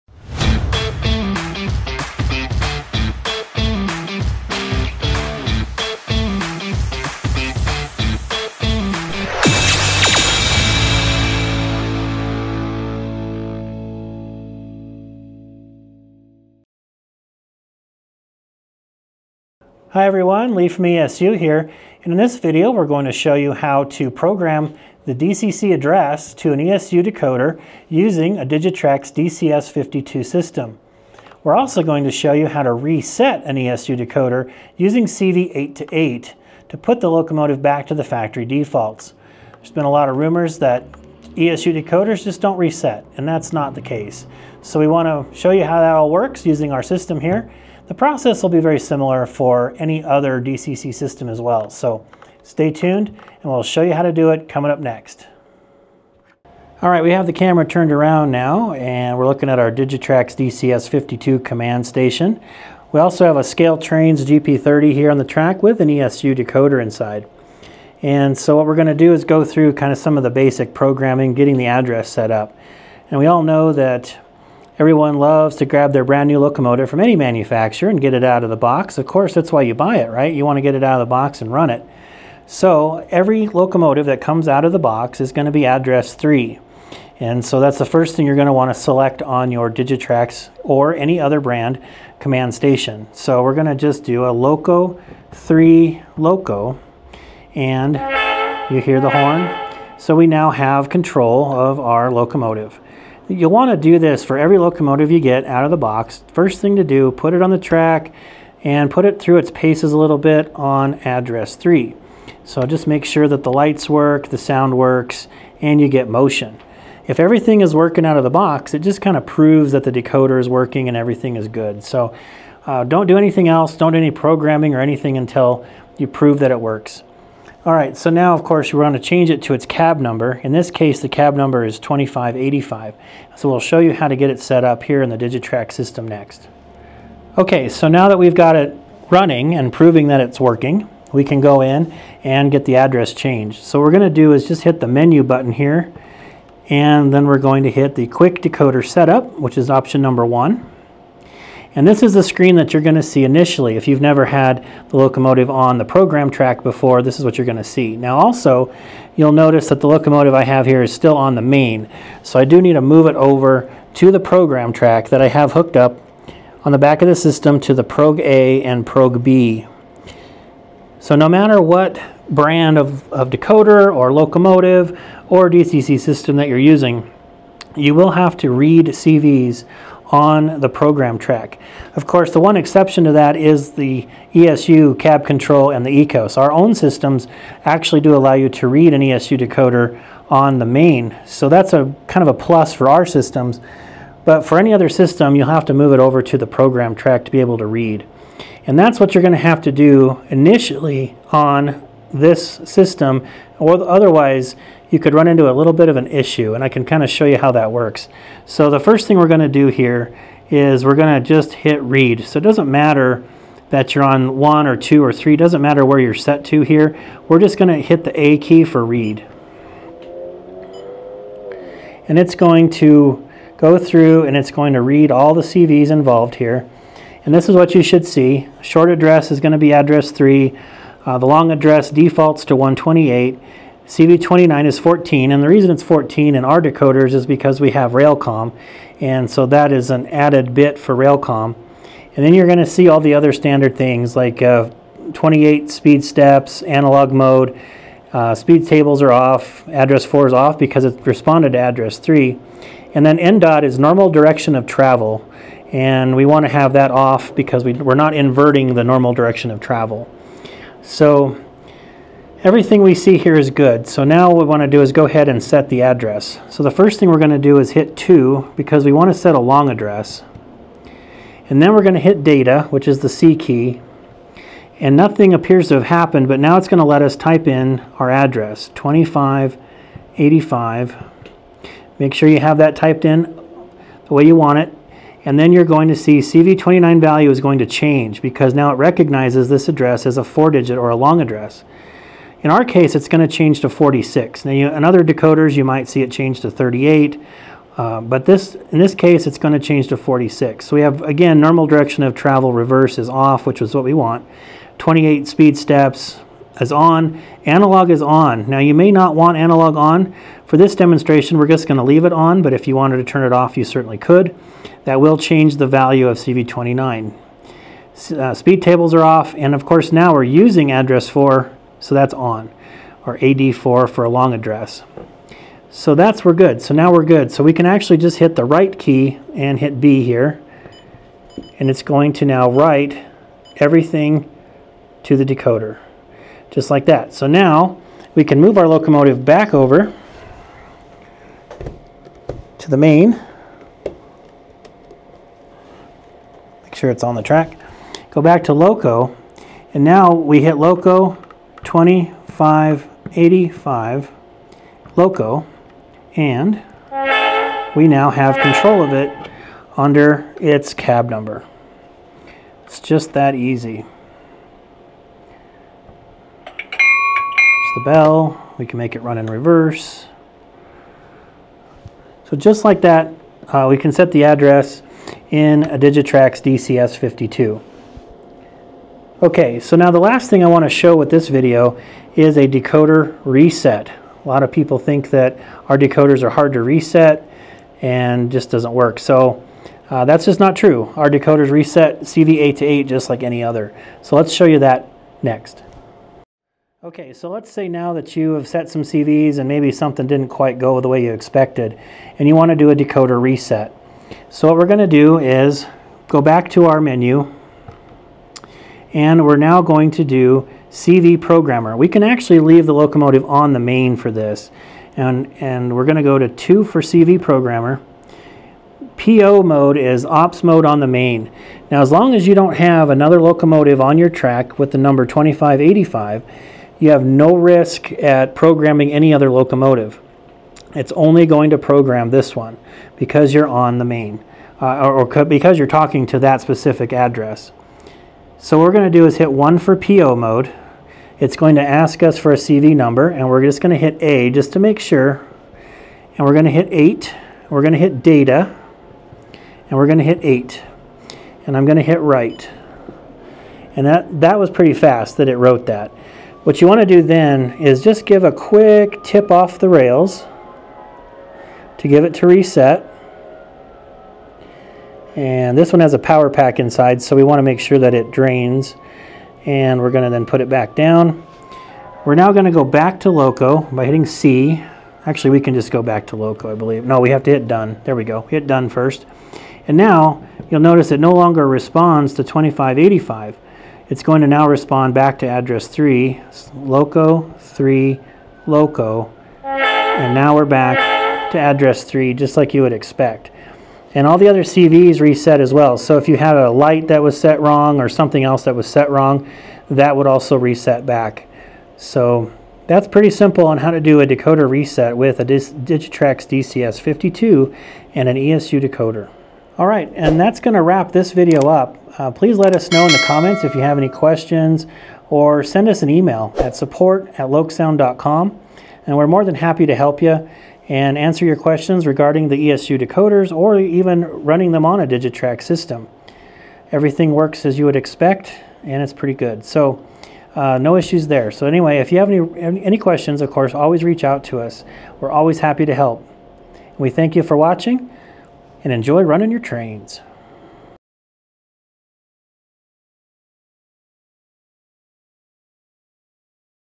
ESU Decoder Programming Basics - DCC Address and Decoder Reset
In this video Lee shows you how to program the address and reset your ESU decoder using your DCC system

Chapters:

0:00 Intro
0:56 Testing Your Locomotive On Address 3
2:17 Programming The Address
6:36 Resetting The Decoder Using CV 8 to 8
8:29 Outro